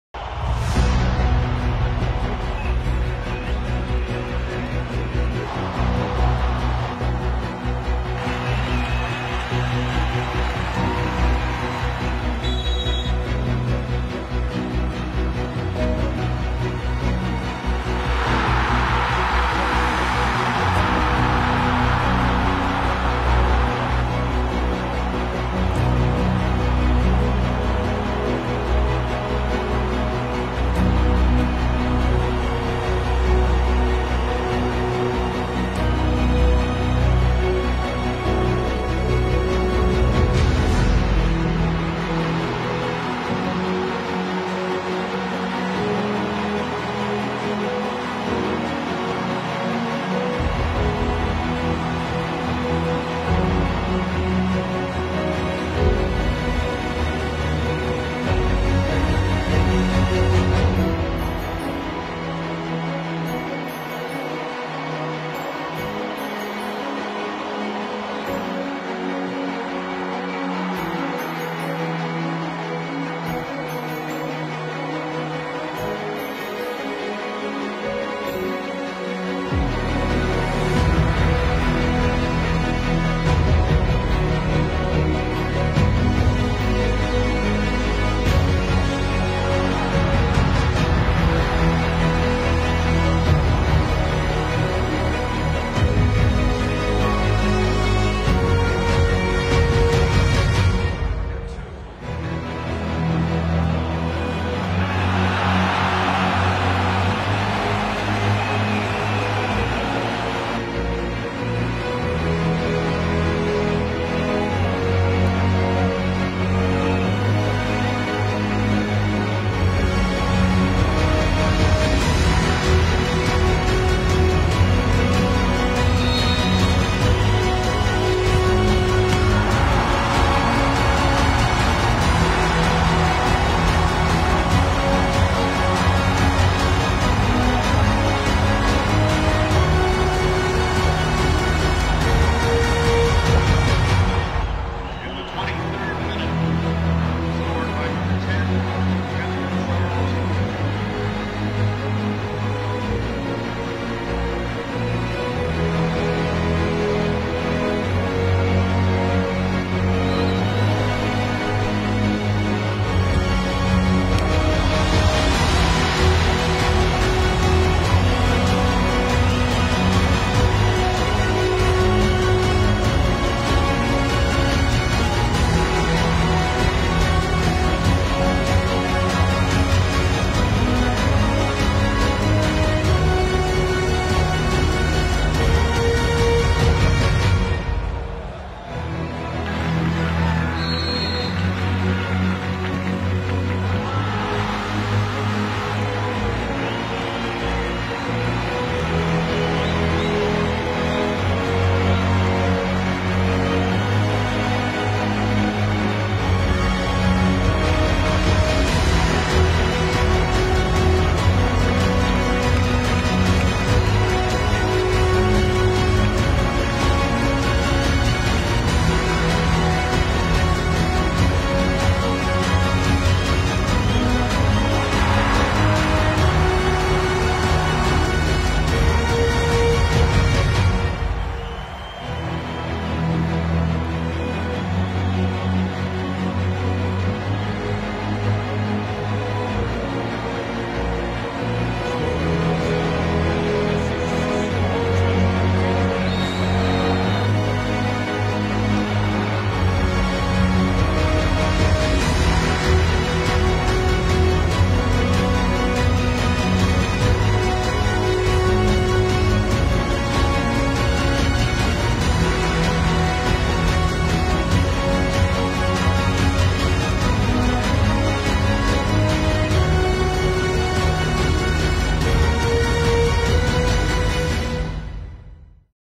⚽️ Most Spectacular Free Kick Goals at the Olympics!
Experience the most spectacular free kicks at the Olympics! This video shows you the best free kicks that have led directly to a goal. Watch the players as Neymar showcase their skills and abilities. Enjoy the incredible creativity that each free kick brings. Don't miss out on the best free kicks of the Olympics!